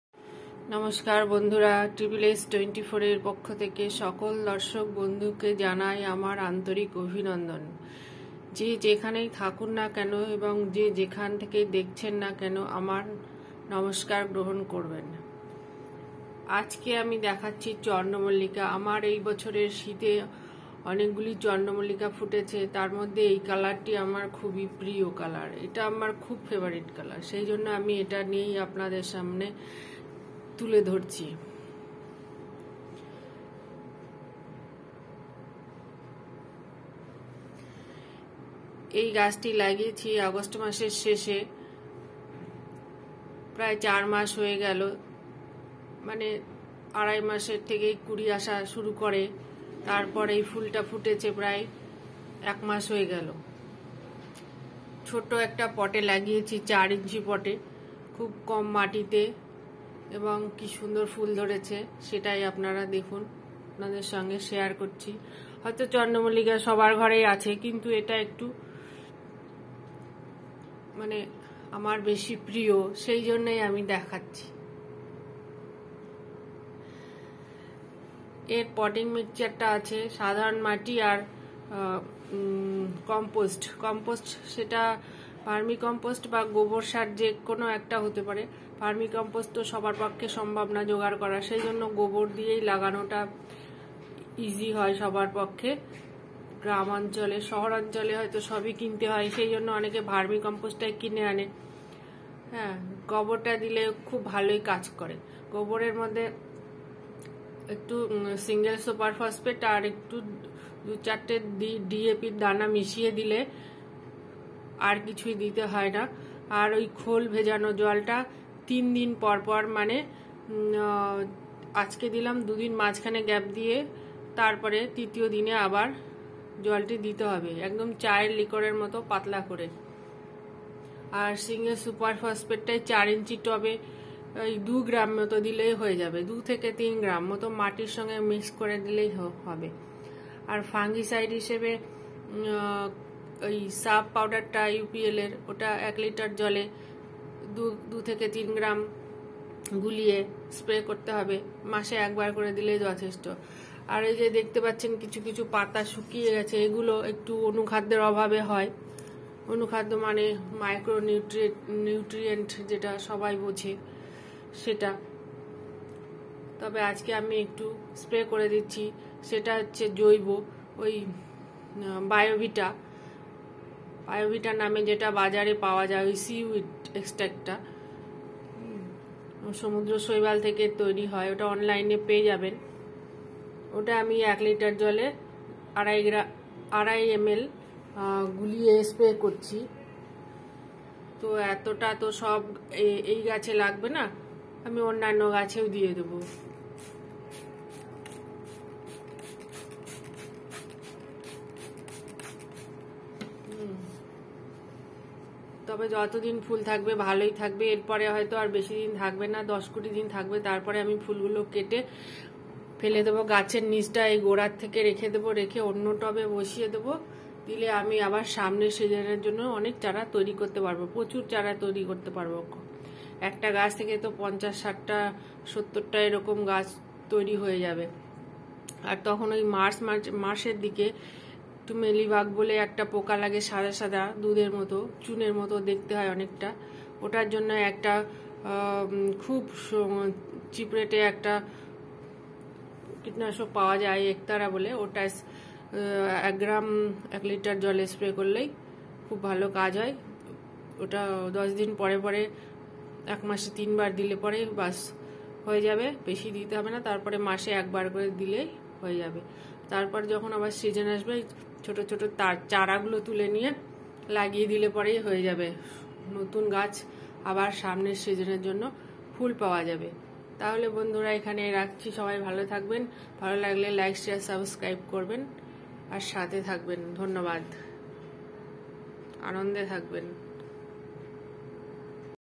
চন্দ্রমল্লিকারA toZ পরিচর্যা how to take care Chandra Mallika flower#youtube
take care of Chandra Mallika and how to propagate next season and used Fertilizer and micro nutrient